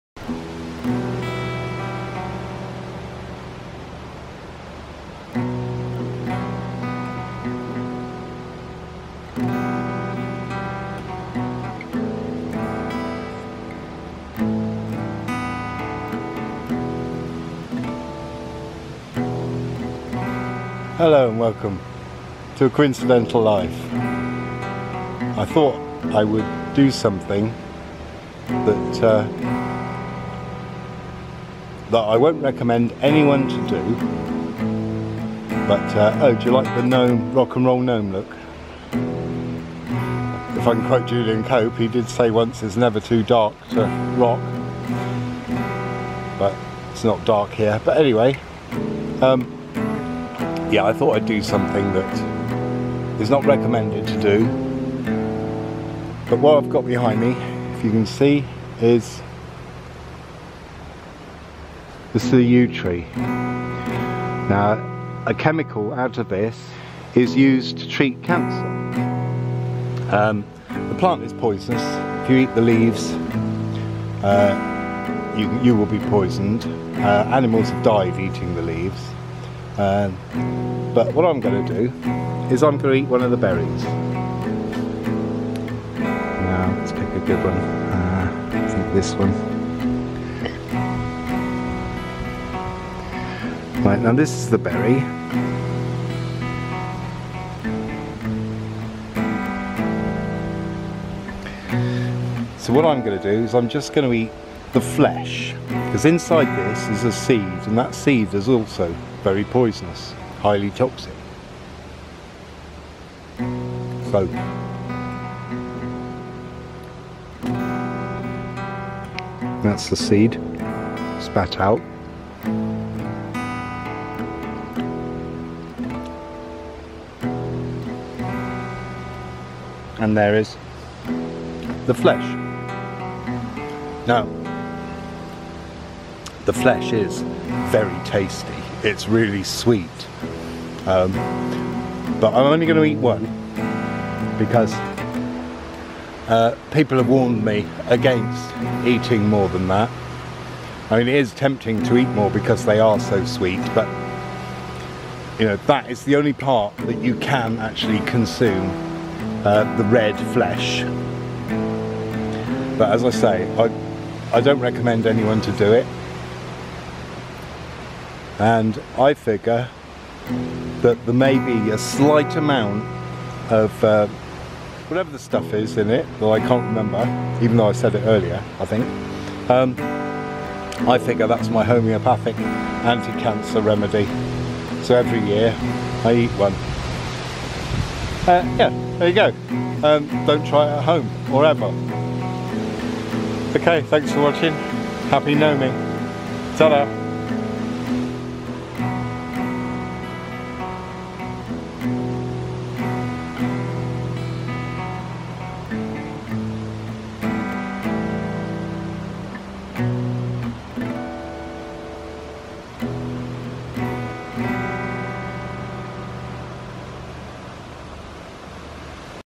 Forbidden Fruit : Yew Berry Eating
The red flesh of yew berries is edible but the seed and foliage isn't. Yew is used to make an anti cancer treatment drug, taxol. Yew is mostly fatal to animals. Don't try this unless you know what you are doing.